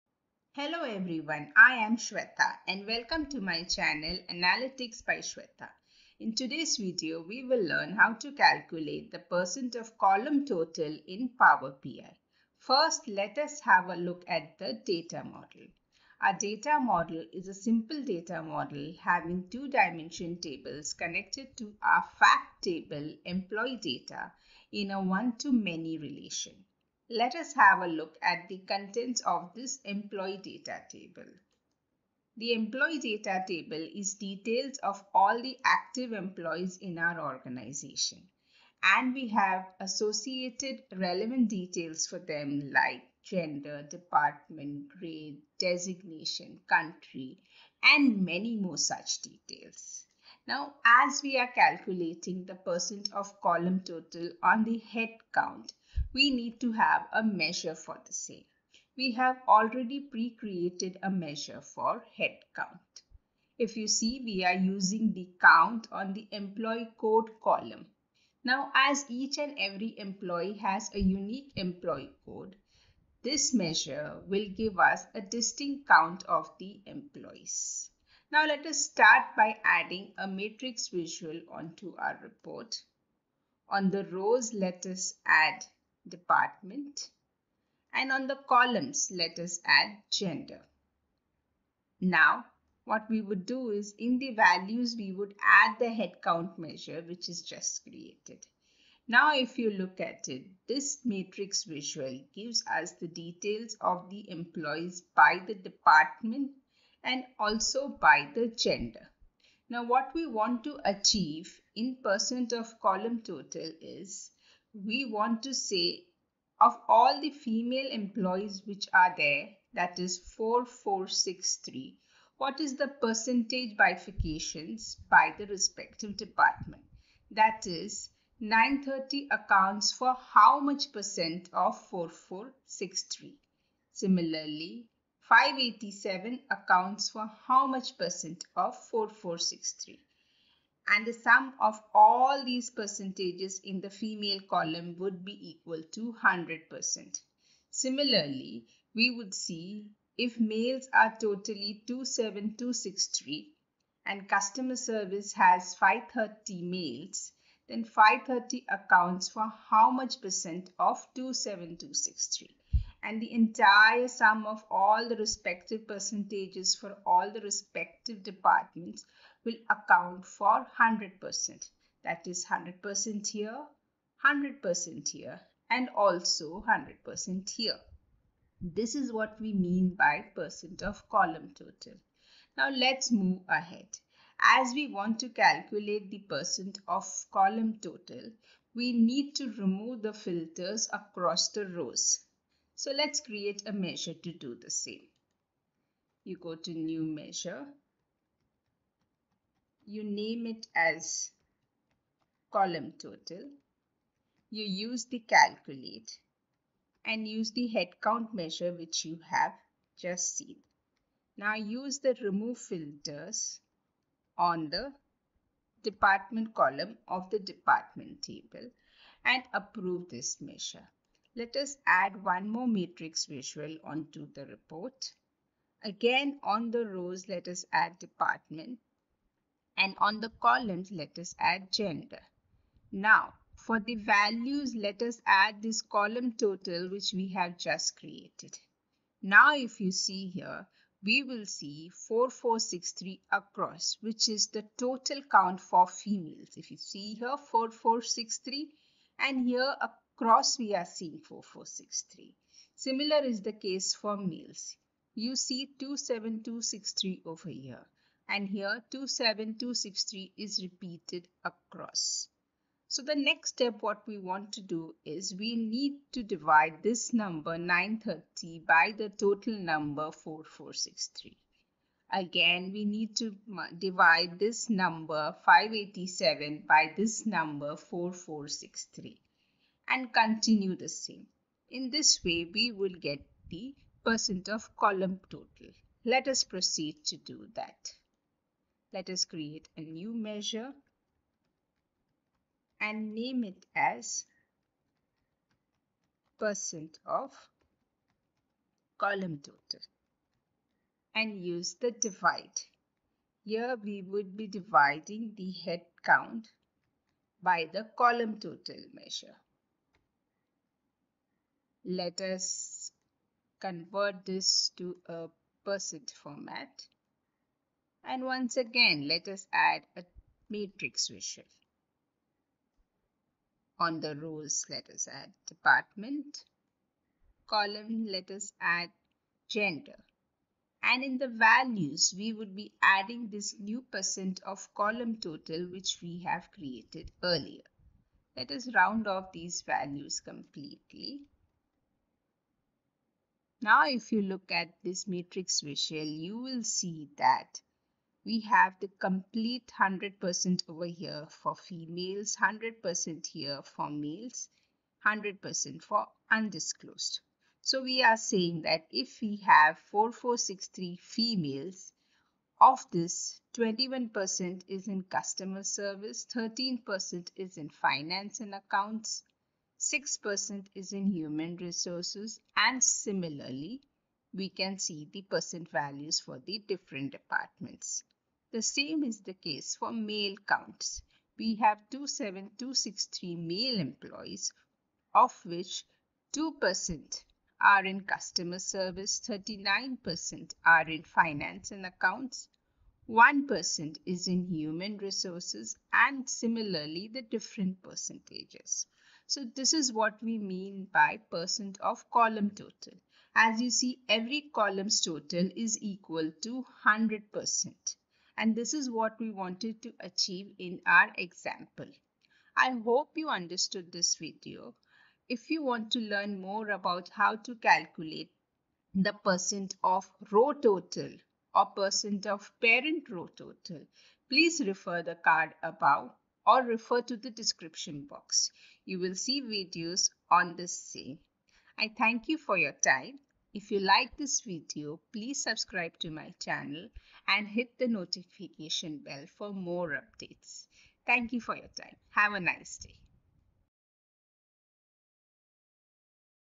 Learn Power BI: Master Percent of Column Total (DAX) | Power BI Beginners | Power BI Tutorials
Learn how to calculate Percent of Column Total in Power BI.

Unlock Your Power BI Skills: Master Column Total % | Mastering Column Total % for Beginners